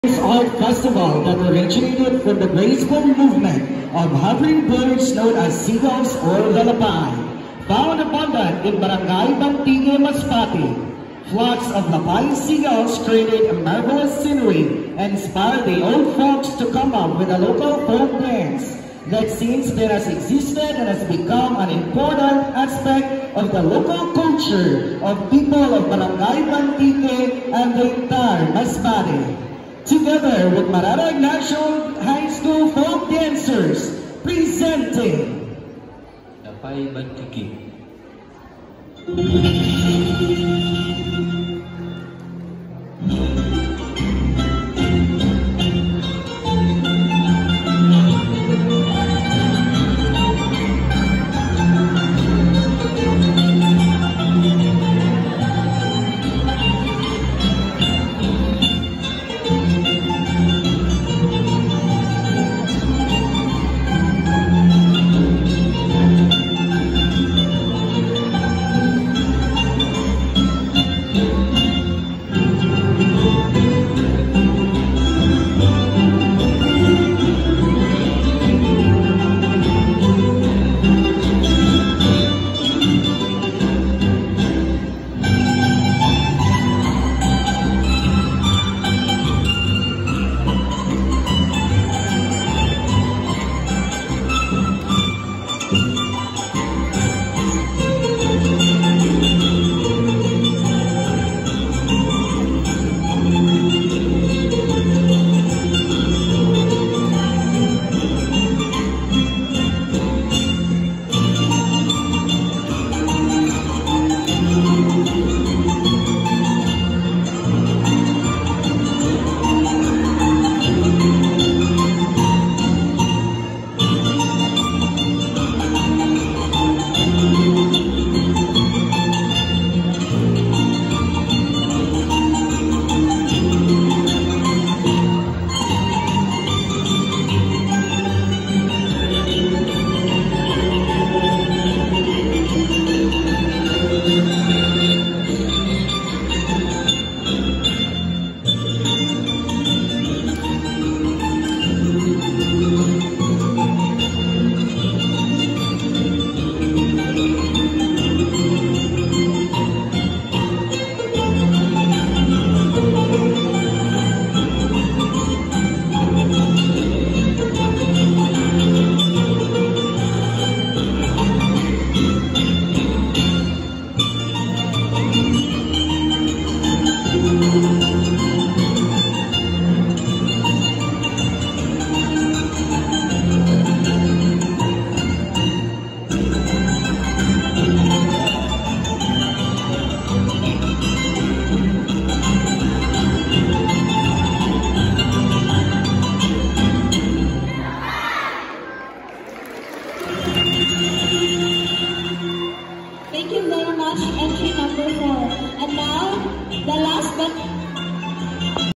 Lapay Bantigue | Mararag NHS
This presentation was performed during the celebration of National Children's Month 2022 where they placed as 3rd.